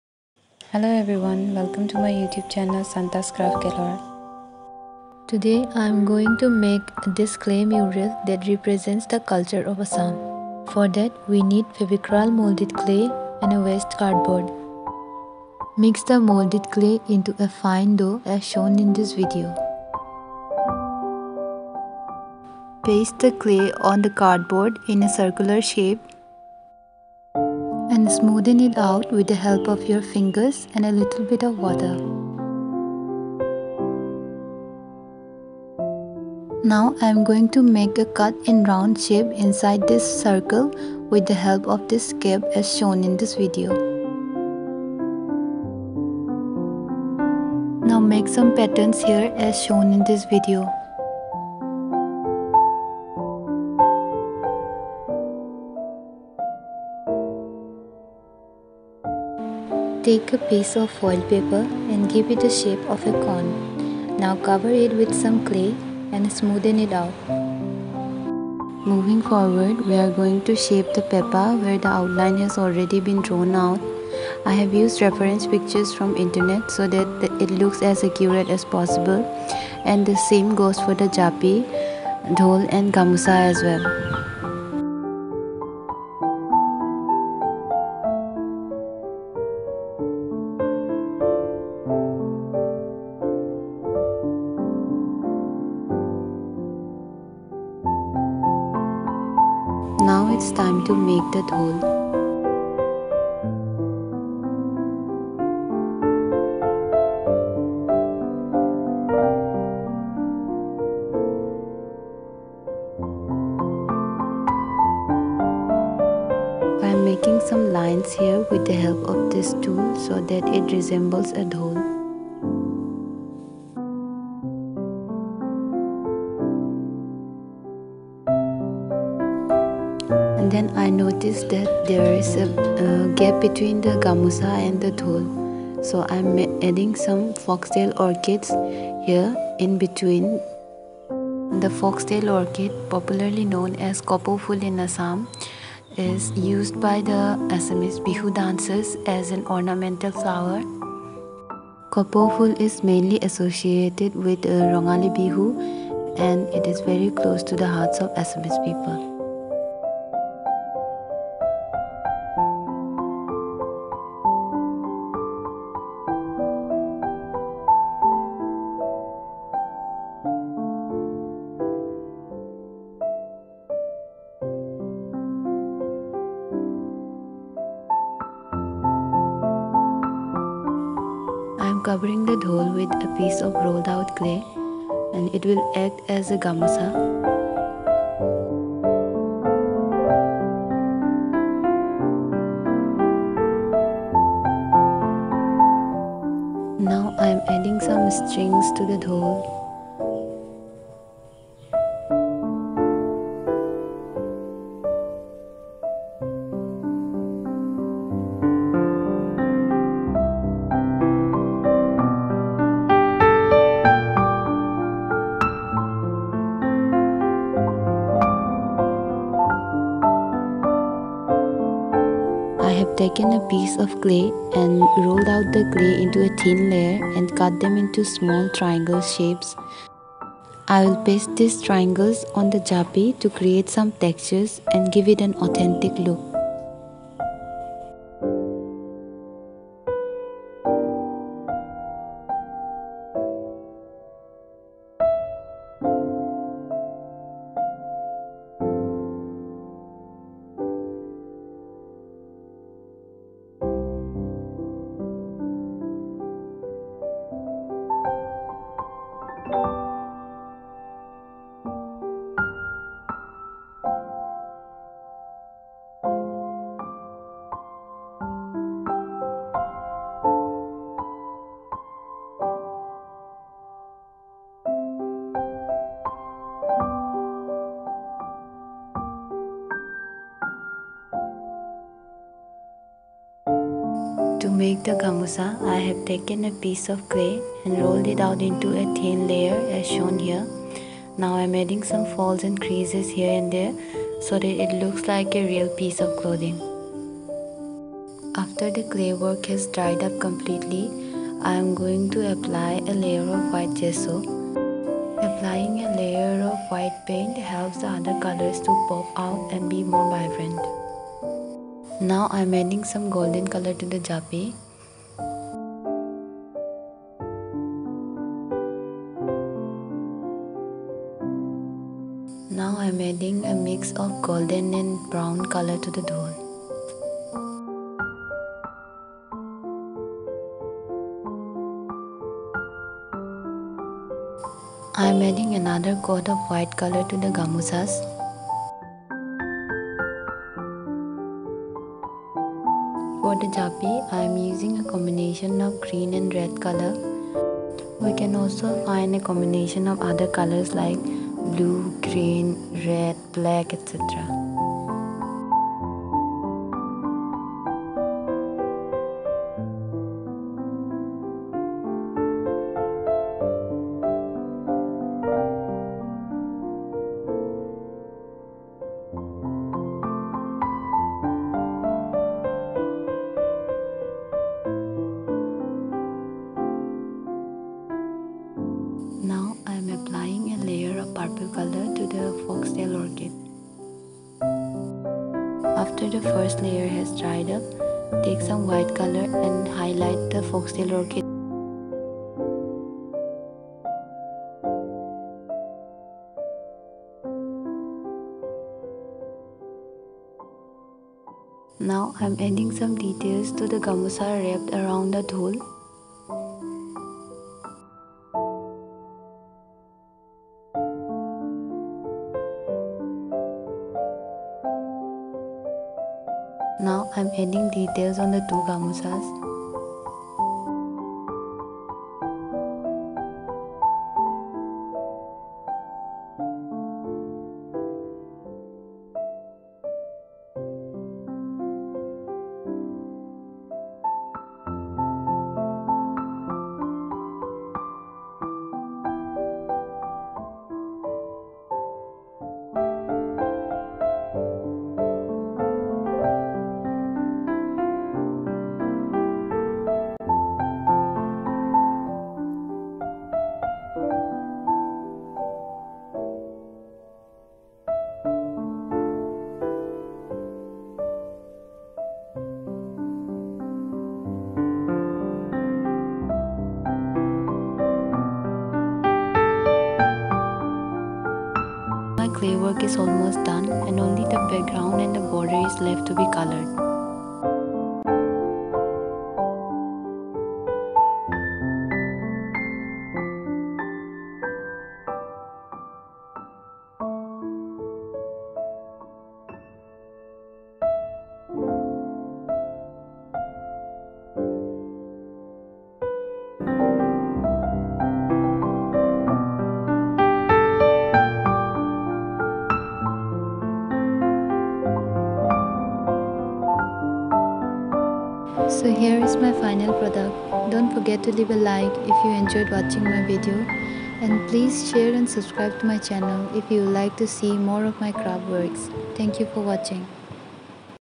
Clay Mural Representing Assamese Culture
This is a clay mural that represents the culture of Assam. The Japi, Gamosa, Dhol, Kopou Phool (foxtail orchid) all are closely related with the culture and lifestyle of Assam. I hope you enjoyed watching the video.